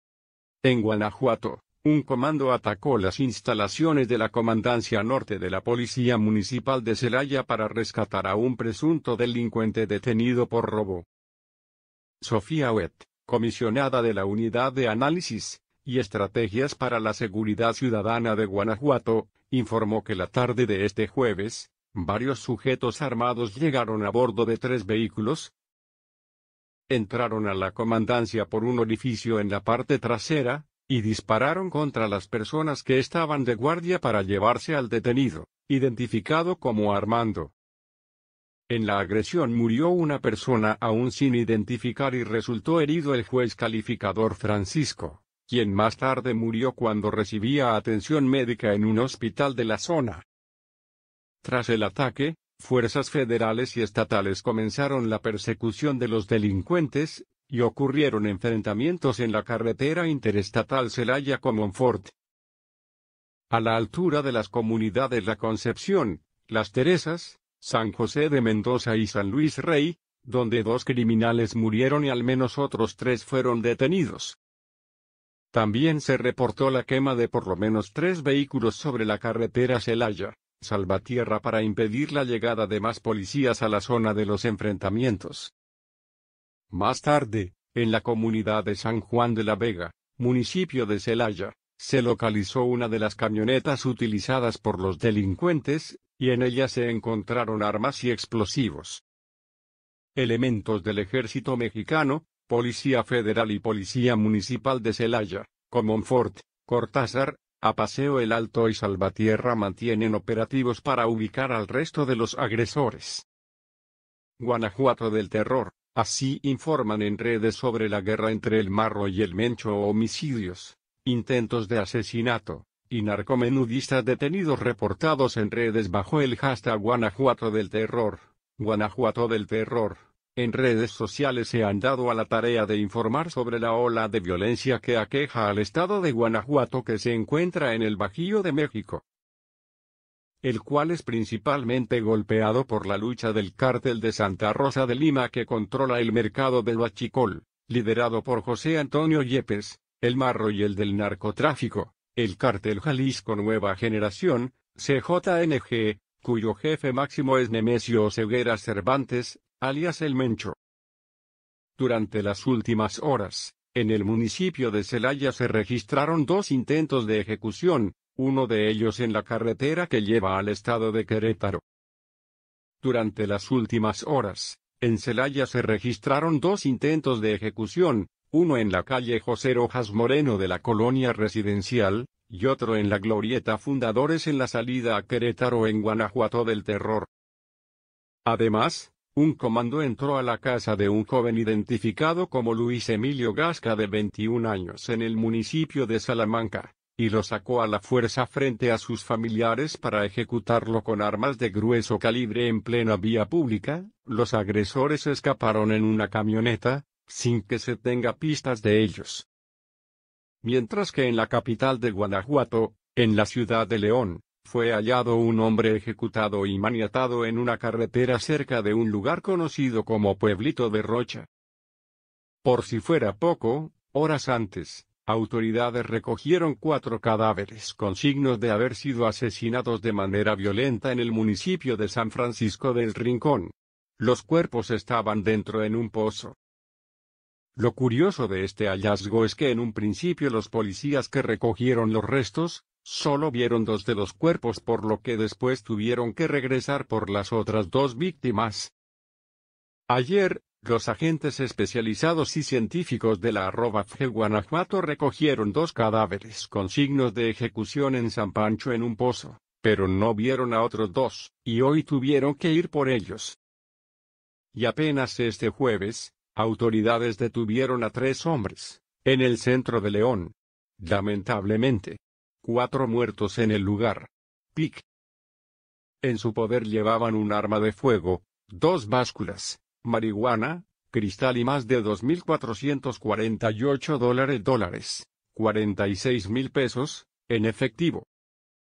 Guanajuato hoy: ataque en Celaya mucha gente MU3RE incluyendo civiles
Guanajuato hoy: ataque en Celaya mucha gente muere incluyendo civiles
El Marro